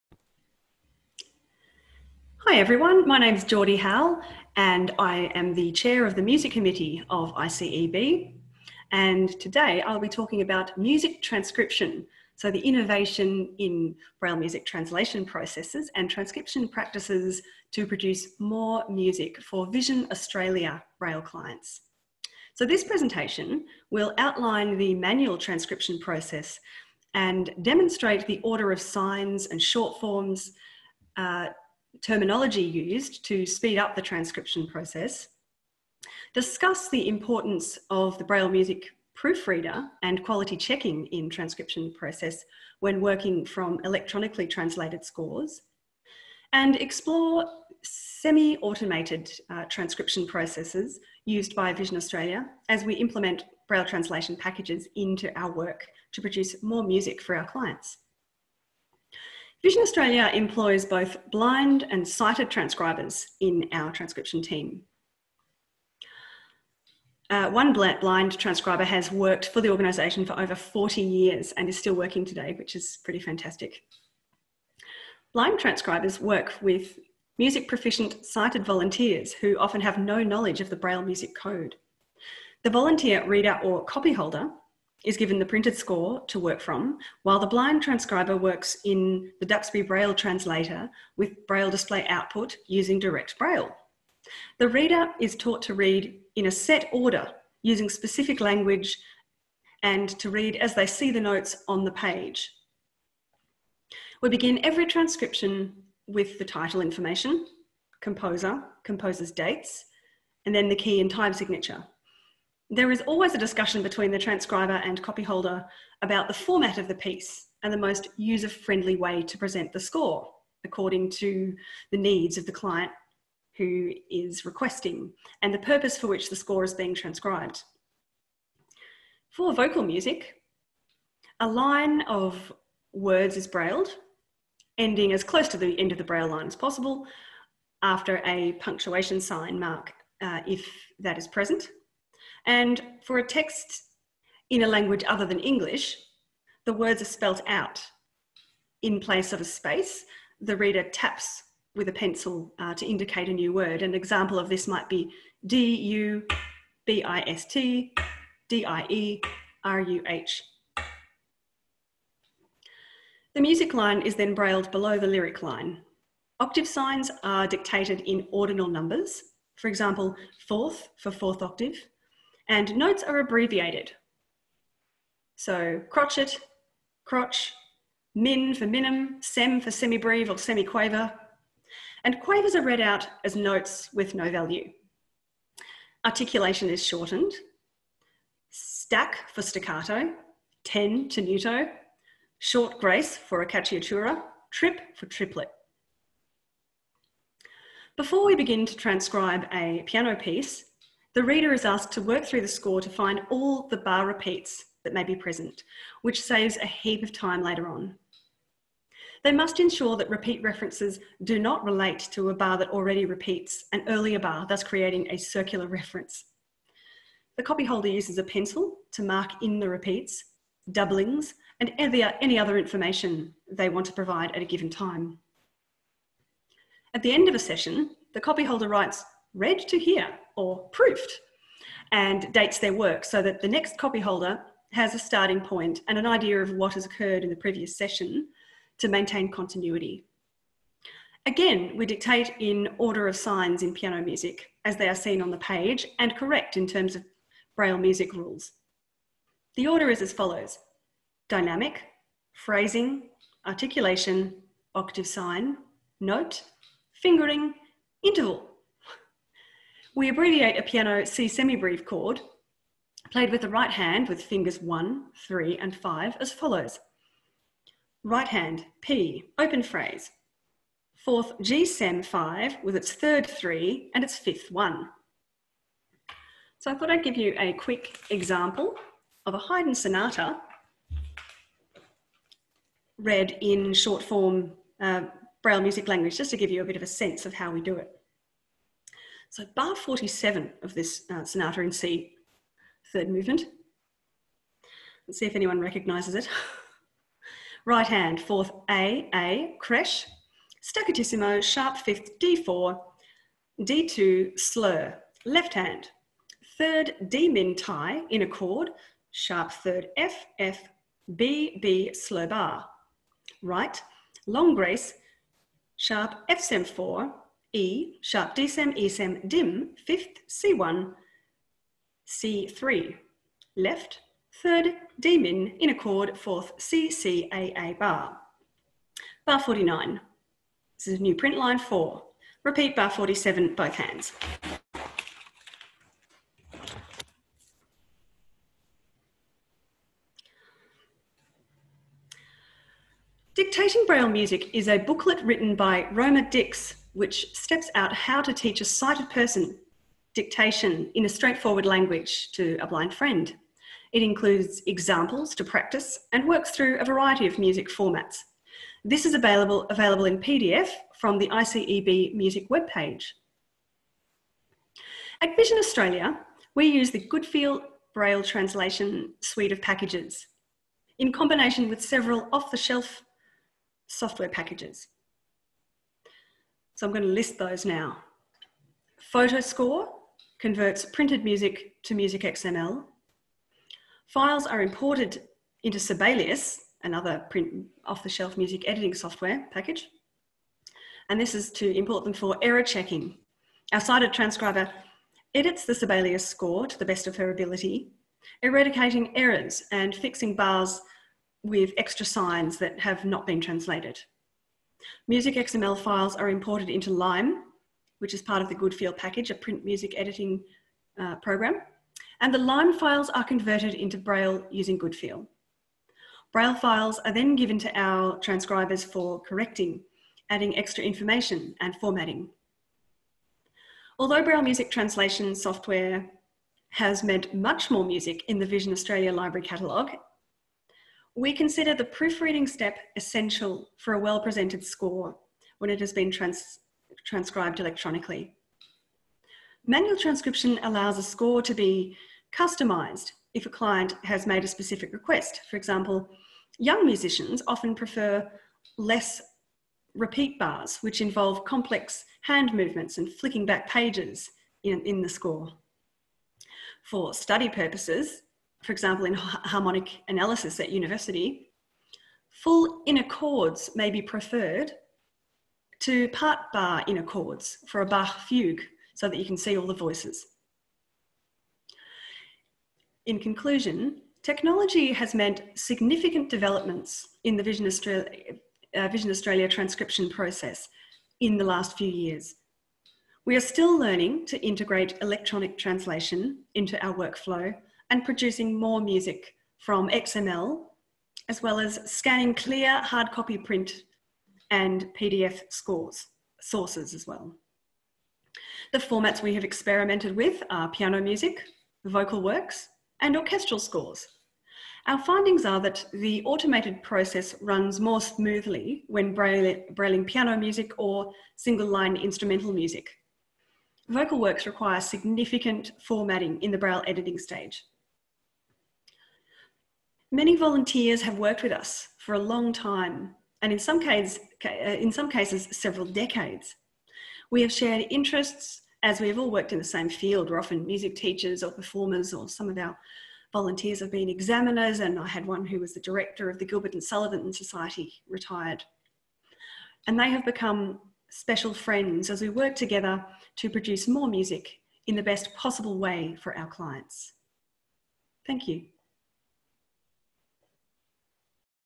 Braille Music Transcription
Innovation in Braille Music Translation Processes and Transcription Practices to Produce More Music
Author: Jordie Howell, Vision Australia

Over the past fifty years music has been transcribed using a variety of methods. Music transcribers who are fully sighted have transcribed on Perkins Braillers or computer from the printed score. Blind transcribers have worked with an amanuensis to produce a) scores from dictation on the slate and stylus, b) Perkins or computer with braille display output, or c) copied out scores from earlier braille editions and made alterations as necessary.

Braille music translation packages have begun to change the landscape for braille transcription internationally. Suddenly, sighted musicians at universities who know nothing about braille can produce legible braille music with the aid of translation, and blind musicians are able to download scores online to run through a braille music translation package for use in rehearsals. They log into the Vision Australia library catalogue, download the brf file onto their braille notetaker which provides options for using the electronic version, or emboss out a hard copy.

Until fairly recently, Vision Australia only produced braille music manually. That is, a blind transcriber worked with a sighted volunteer who is proficient in print music, and dictated the score. This method is still in place, and the process has been refined over the years with a shorthand terminology that makes the process more efficient. However vision Australia also employs a sighted transcriber who works both manually and electronically. Print music editing software (Sibelius and photoscore) are used to achieve good quality scanned images which convert to print music notation. These are then translated through the Dancing Dots suite of programs to convert to braille music.

This presentation will:
* Outline the manual transcription process and demonstrate the order of signs and shorthand terminology used to speed up transcription;
* Discuss the importance of the braille using proofreader and quality checking in the transcription process when working from electronically translated scores;
* Explore semi-automated transcription processes used by Vision Australia as we implement braille translation packages into our work and produce more music for our clients.